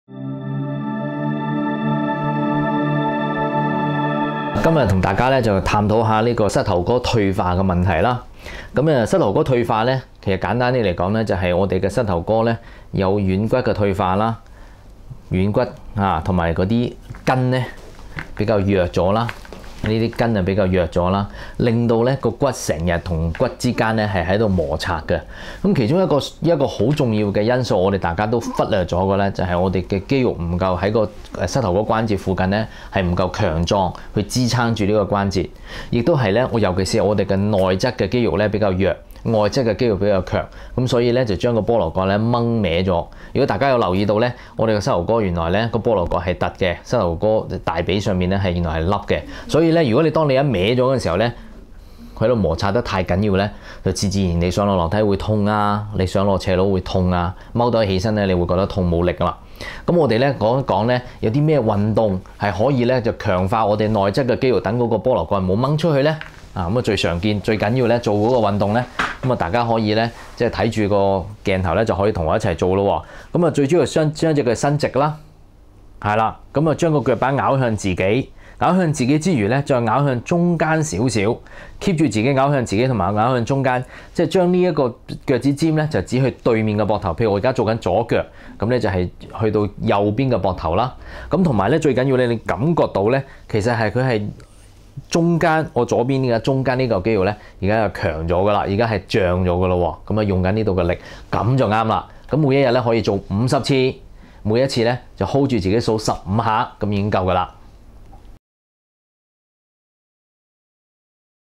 SeeHealth@HTV 小貼士 林敬熹醫生 膝頭退化
林敬熹醫生介紹膝頭退化的小貼士